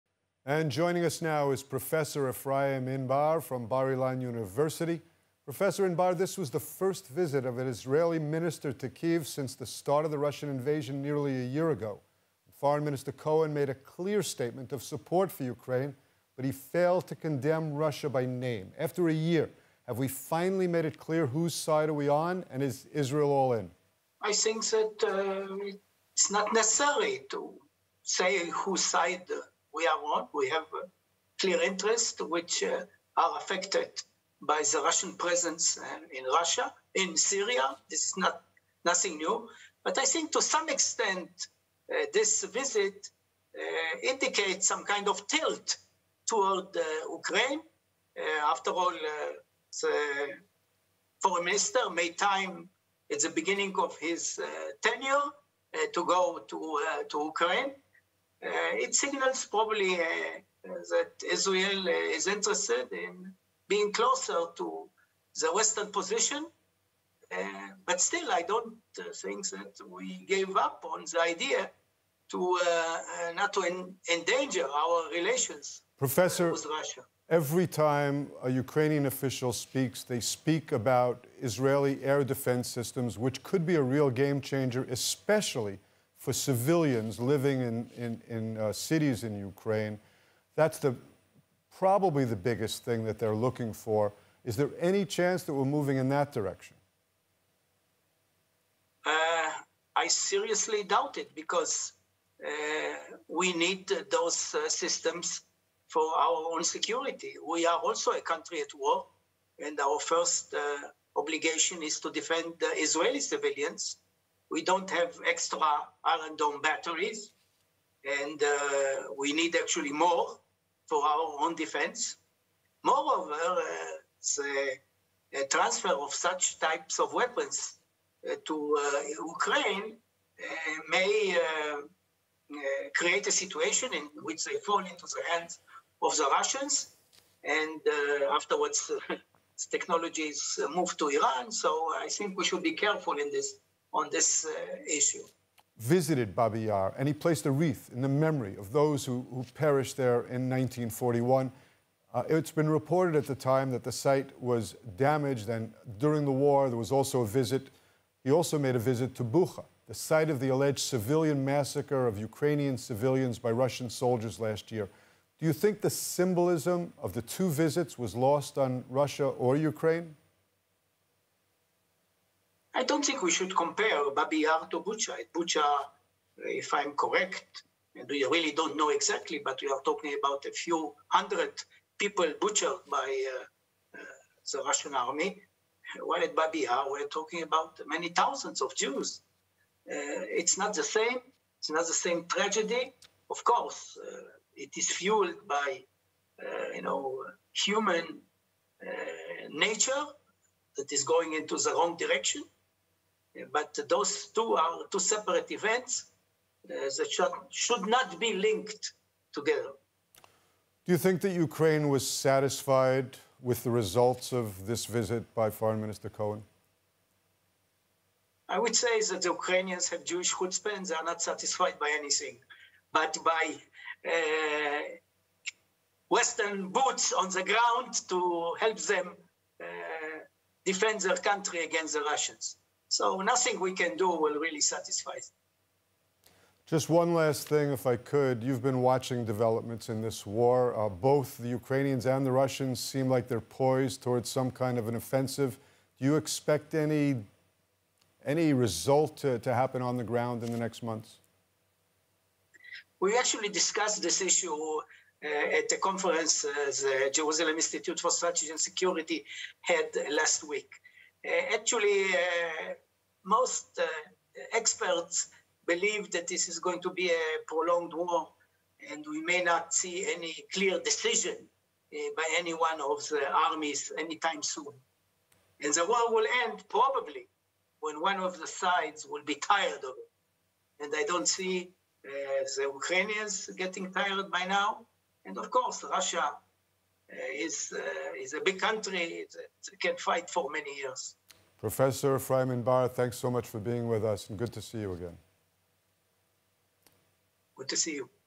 discuss in depth about Israel's Foreign Minister Cohen’s trip to Ukraine
Prof. Efraim Inbar president of the jerusalem institute for strategy and security joins ILTV to discuss in depth about Israel's  Foreign Minister Cohen’s trip to Ukraine and the complexity of the matter